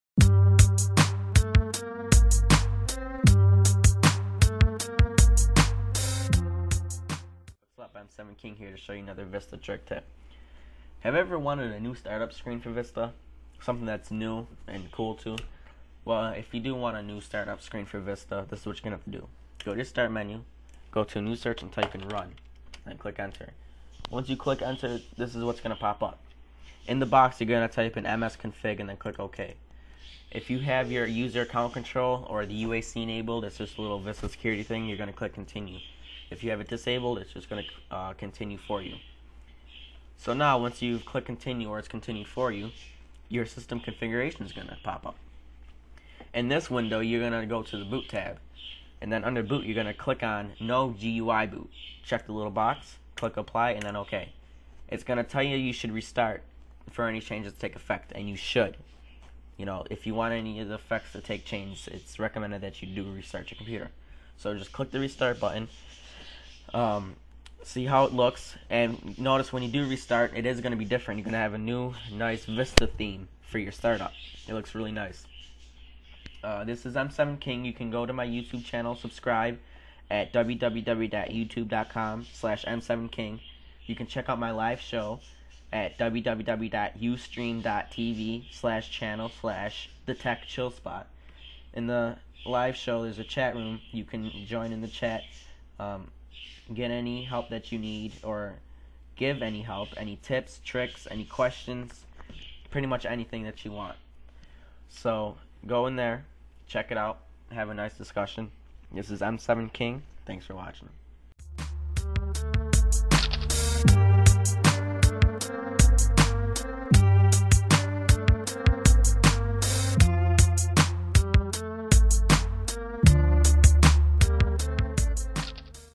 Change your start up screen for windows vista
This video explains how to get a cool vista theme for windows vista start up screen.